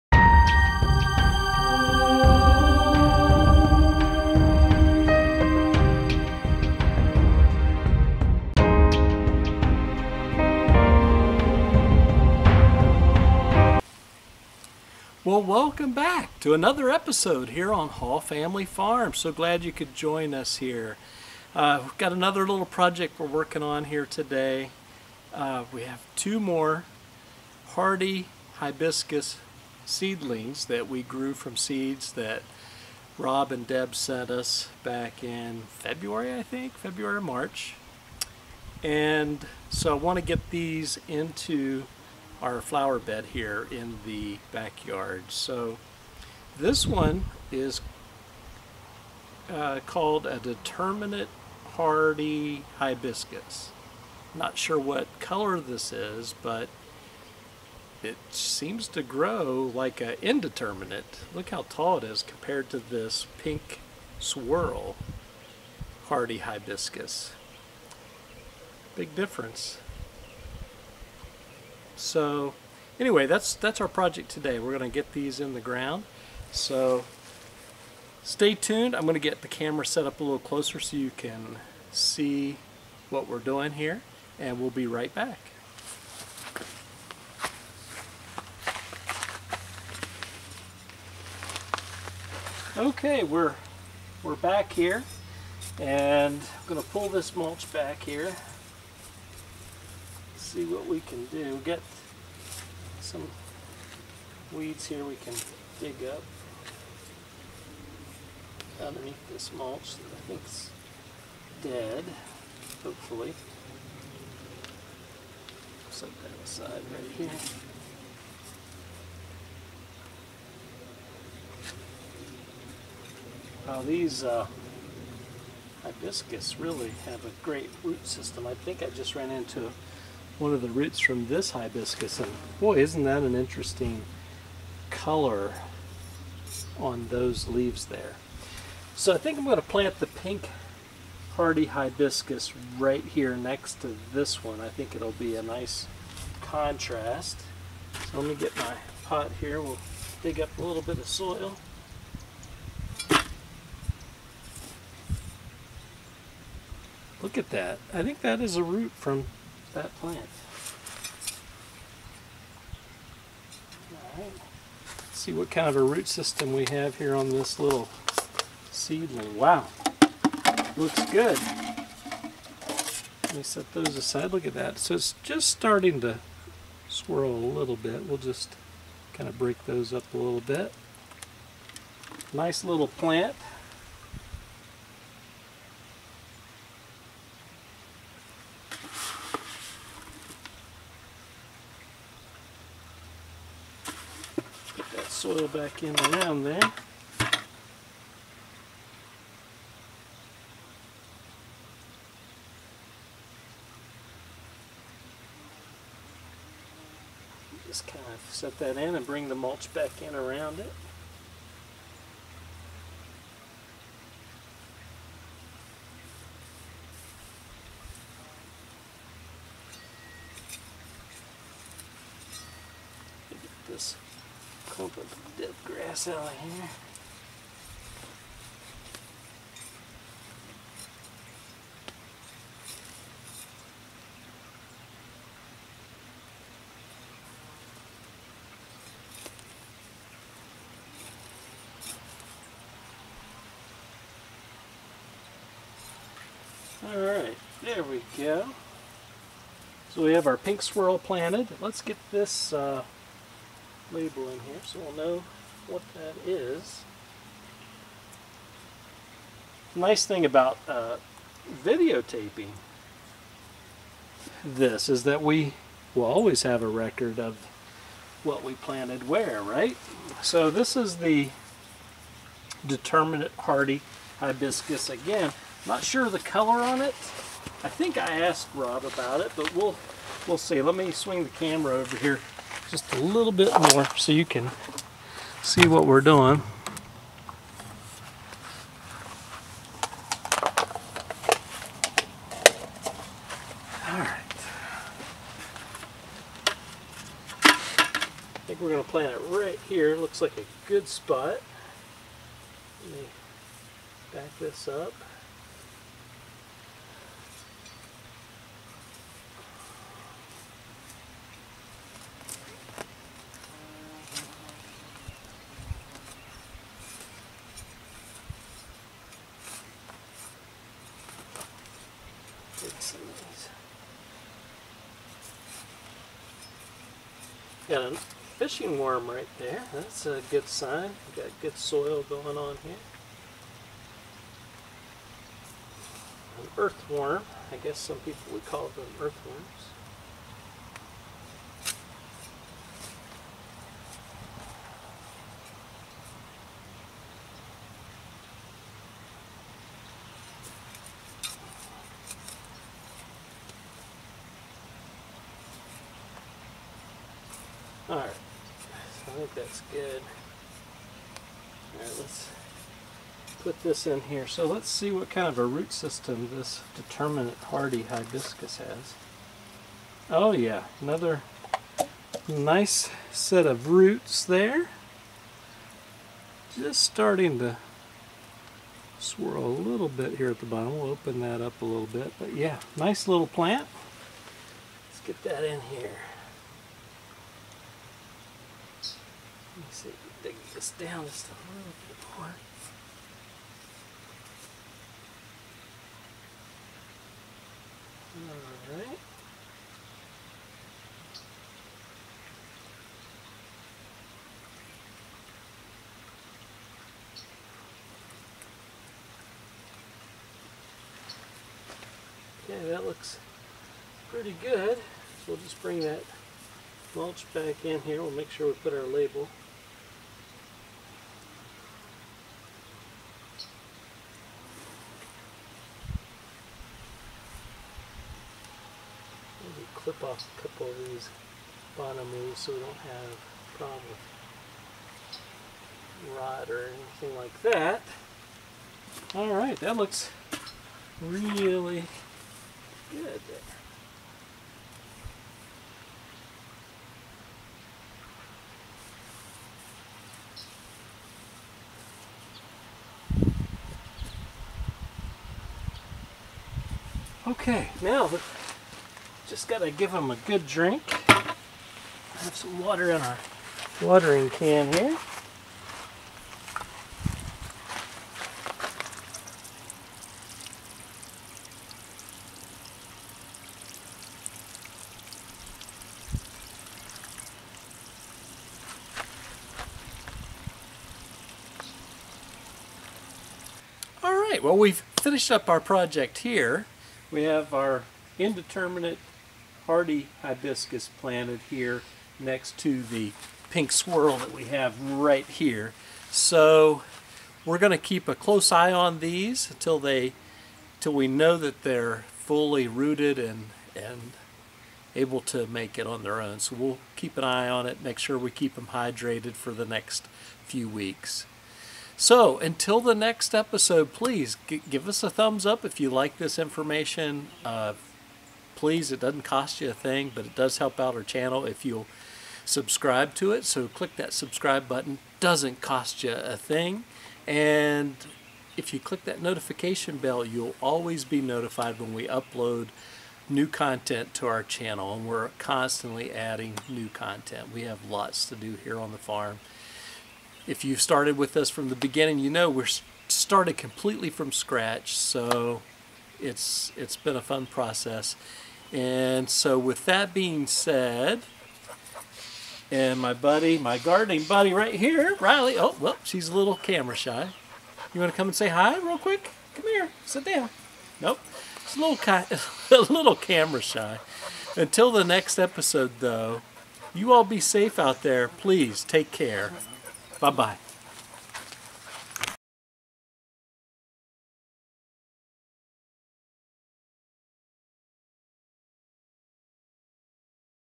Planting Hardy Hibiscus Plants I Grew From Seeds: Part 2. Episode 42
Another huge shout out to my good gardening friend, Rob Cutlip, for collecting seeds from his hardy hibiscus plants and mailing them to me to grow in my landscape!